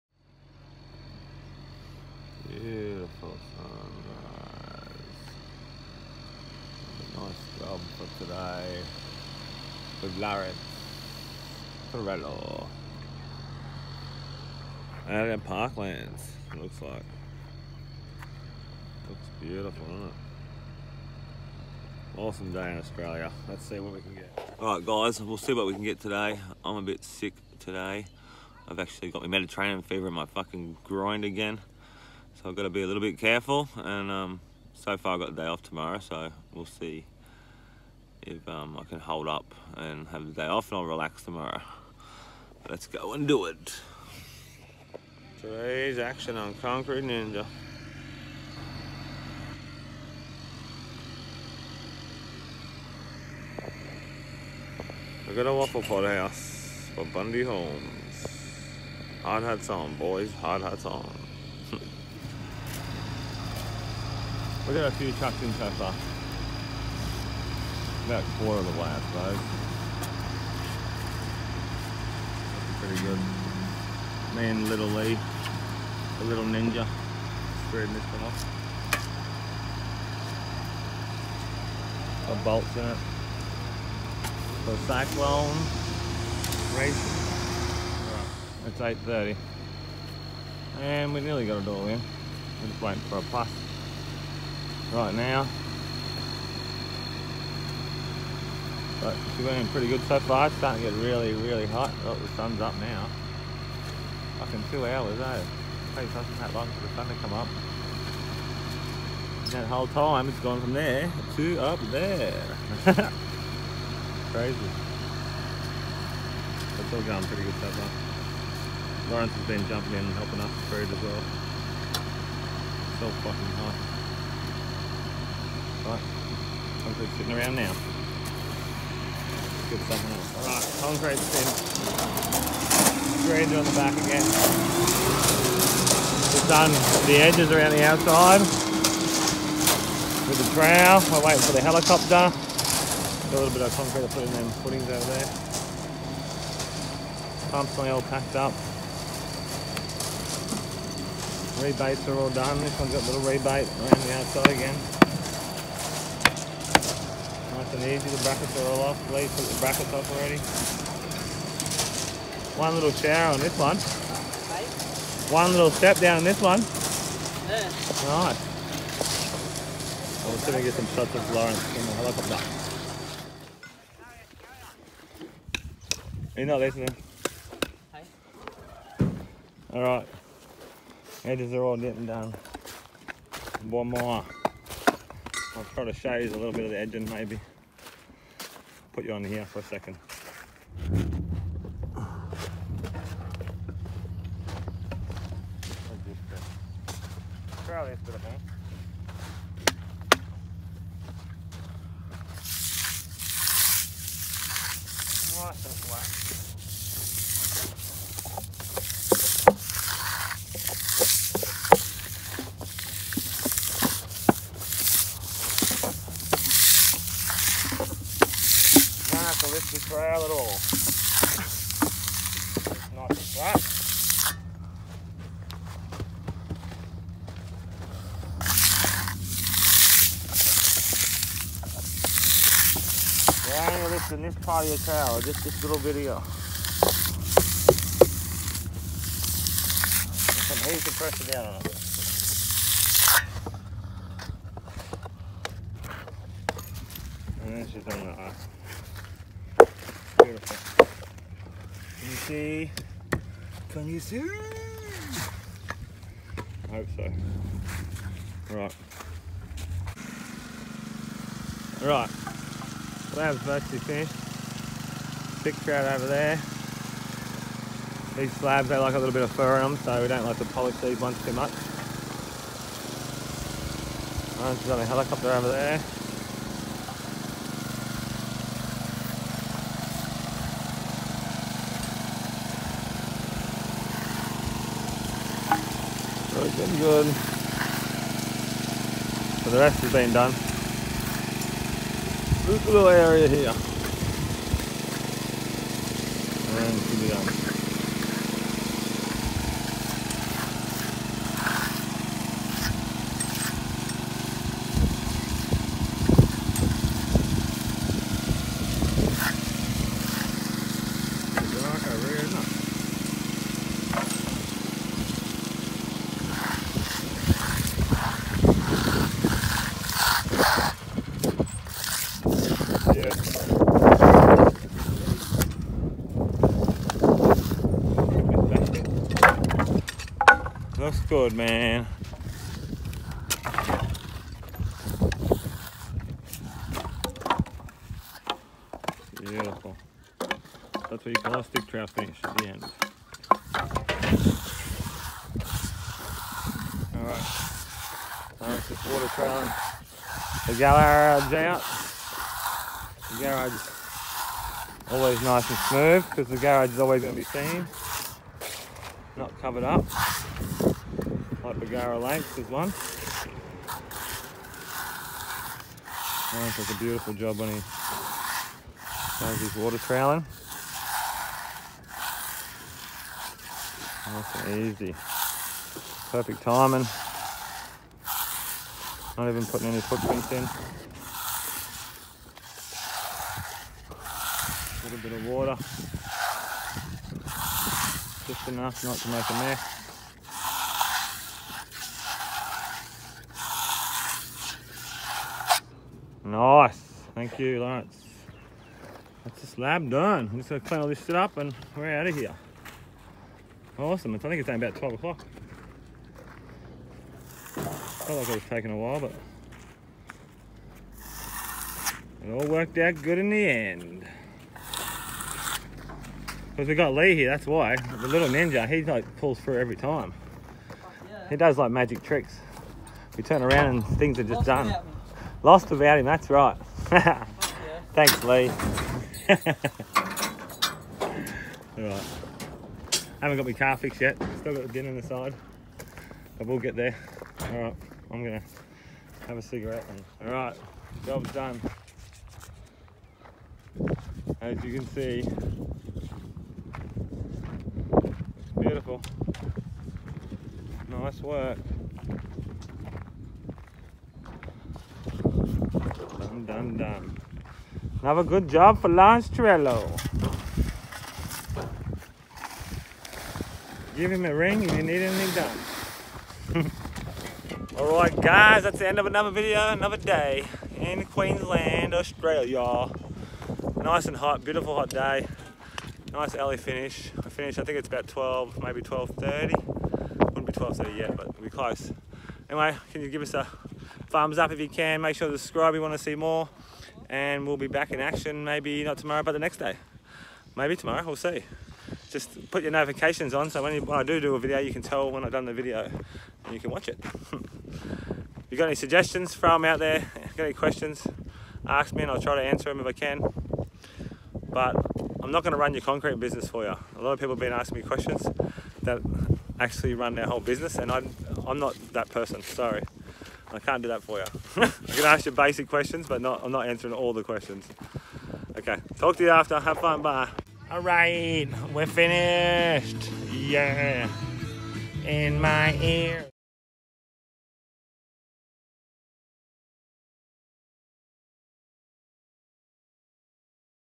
How to use a concrete trowel
Nice hot day concreting in Queensland Australia in this video we give you some tips on how to use a trowel and also water wiping .